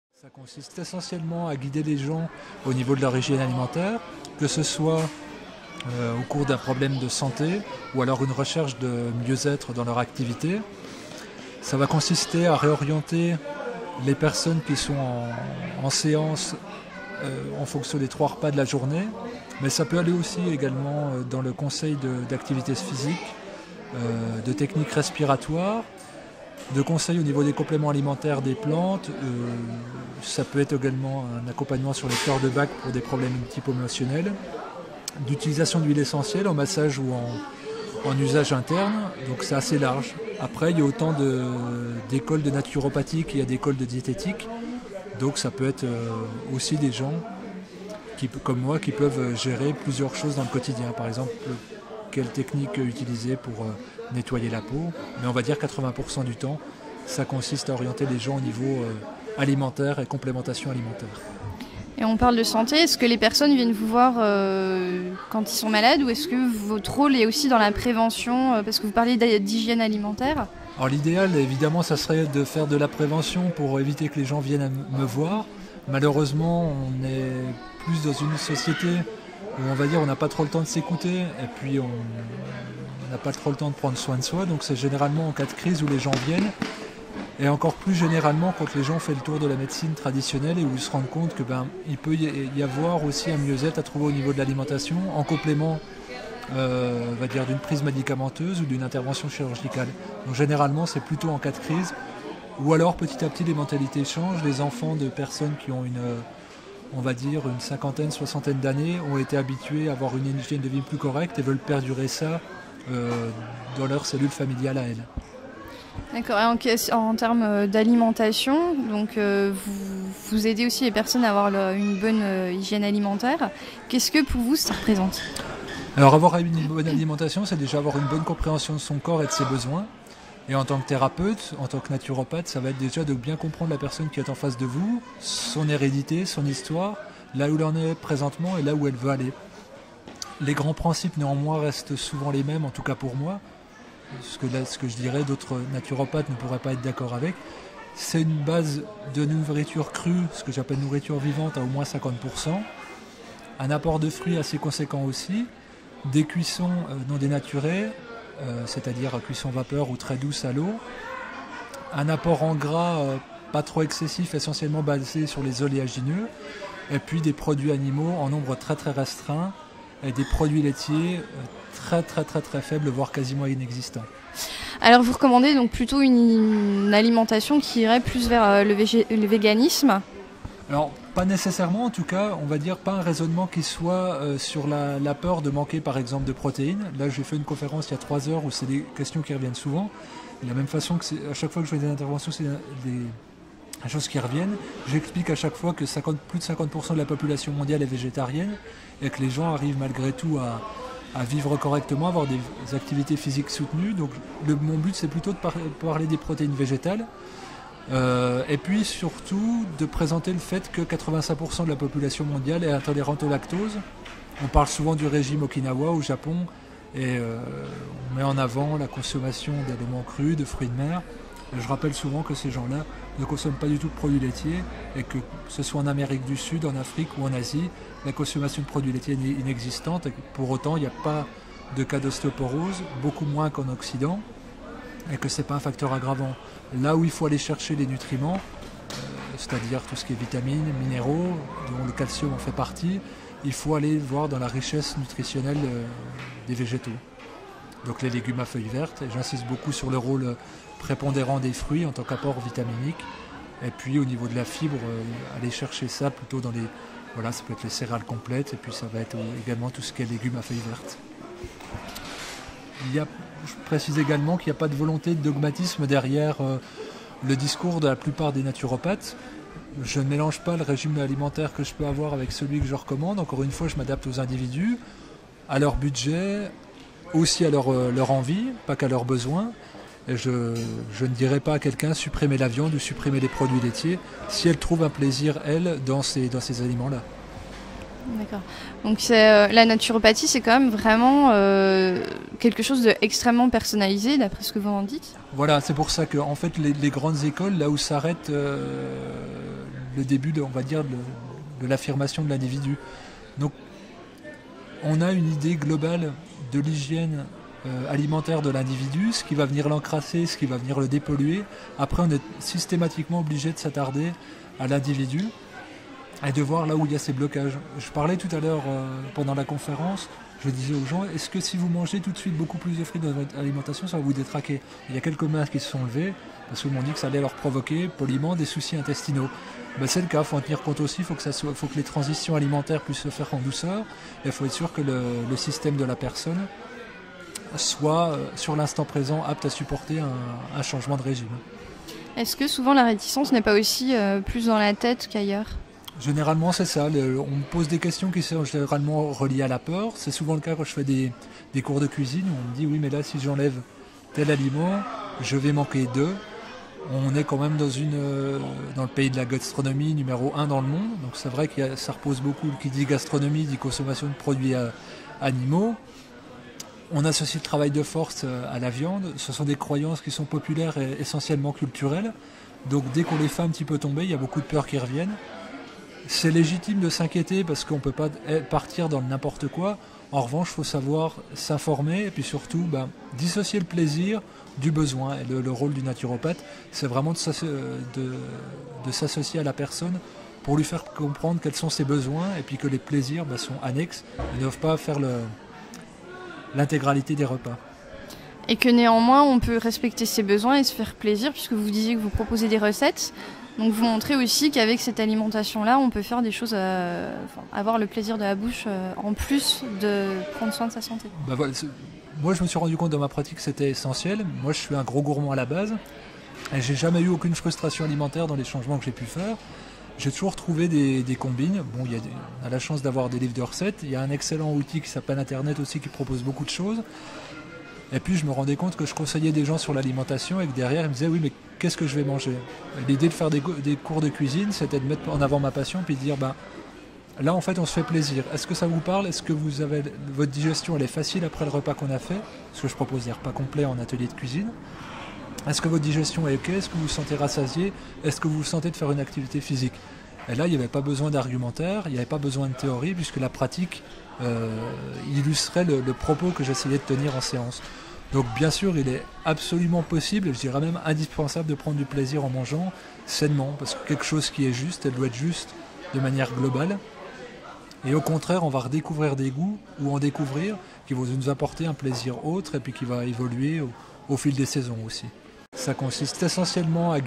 Qu'est-ce qu'un naturopathe? Interview (Fabien Moine)
Interview de Fabien Moine (naturopathe) réalisée pour Radio Campus Besançon.

Nutrinamie, chaîne du projet Nutrinamie, animée par Fabien Moine, naturopathe. Informations d'hygiène et de santé : naturopathie, hygiènisme, alimentation vivante, sans gluten.